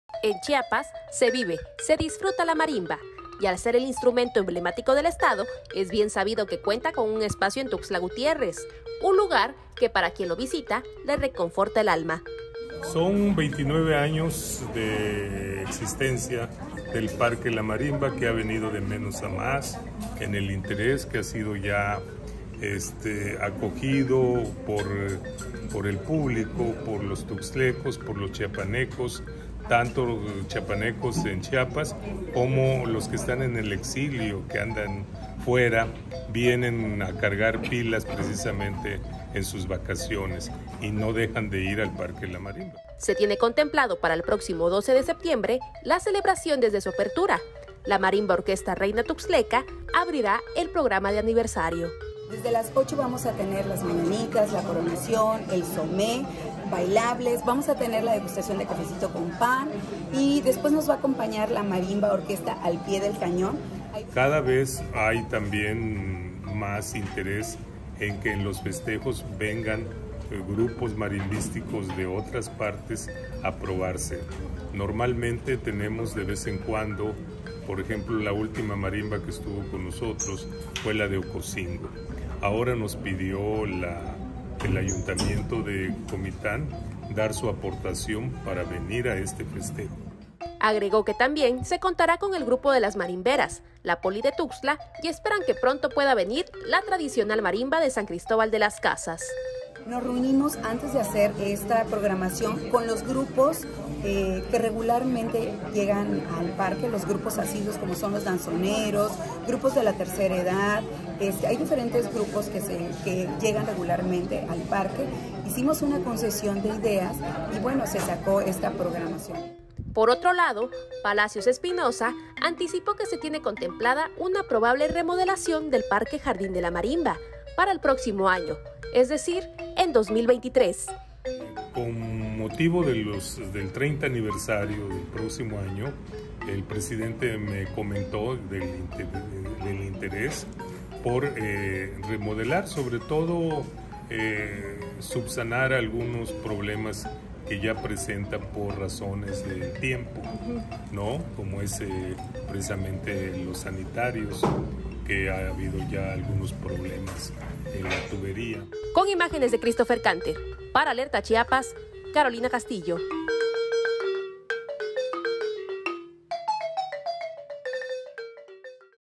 El parque de la Marimba cumplirá 29 años
La Marimba Orquesta Reina Tuxtleca abrirá el programa de aniversario el próximo 12 de septiembre.